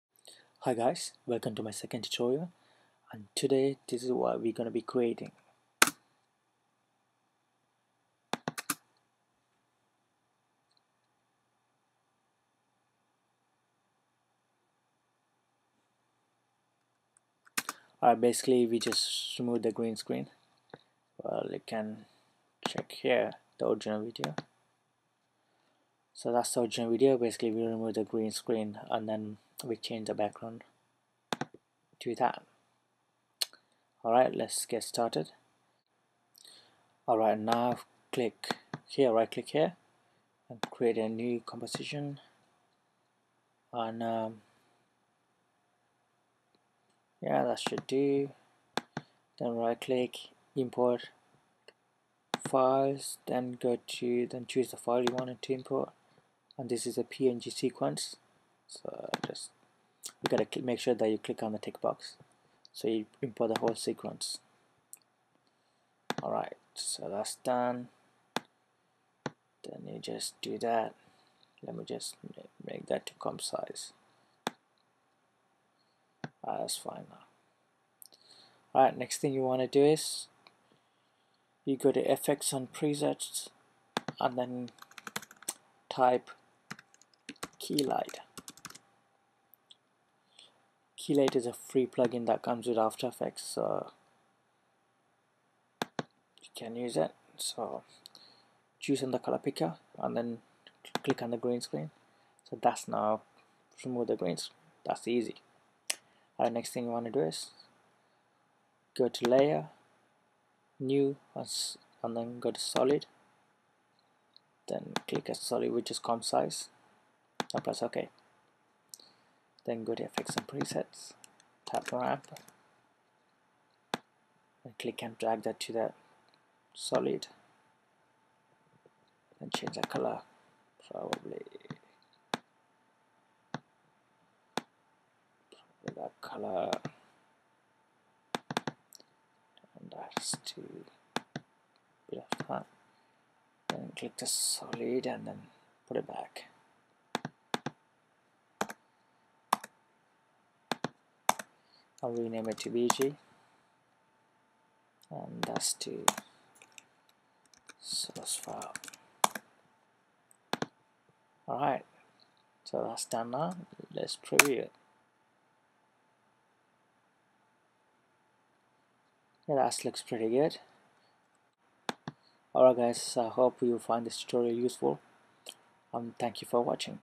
Green Screen - After Effects Tutorial
In this tutorial, I will show you how to remove green screen backdrop.

Here is link for downloading the source video.